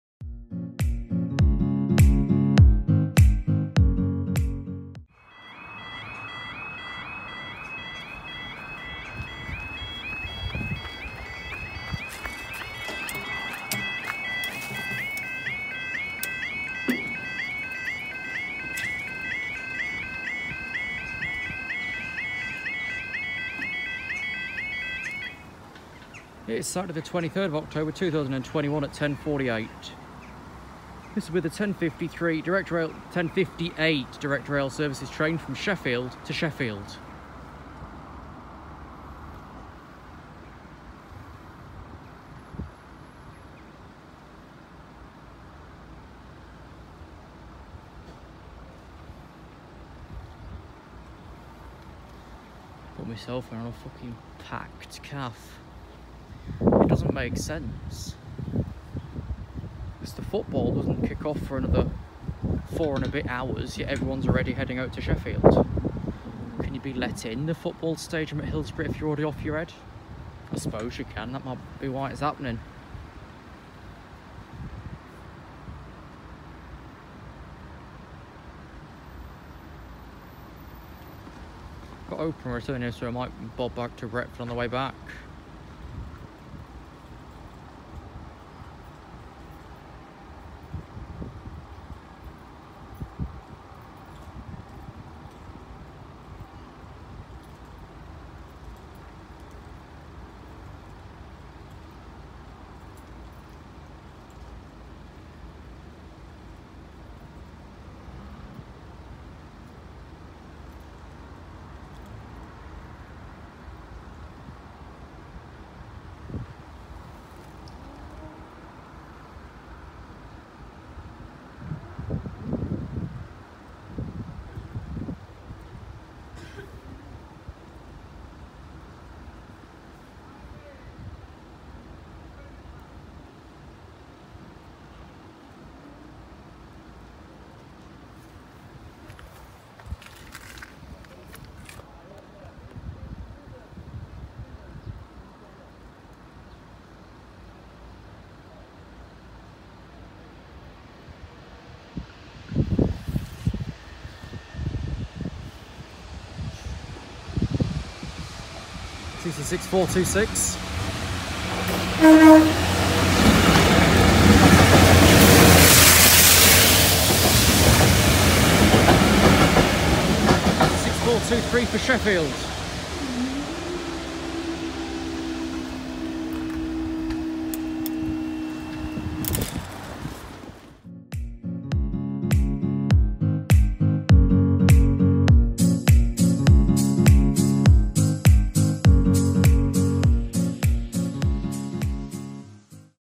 Worksop Level Crossing (23/10/2021)
Recording Location: B6045 - Carlton Road, Worksop, Nottinghamshire
Postcode: S81 7AA
Timetable: Worksop (WORKSOP)
Trackside Information:  MAC3 56 Miles 66 Chains.
Line Speed: 60mph.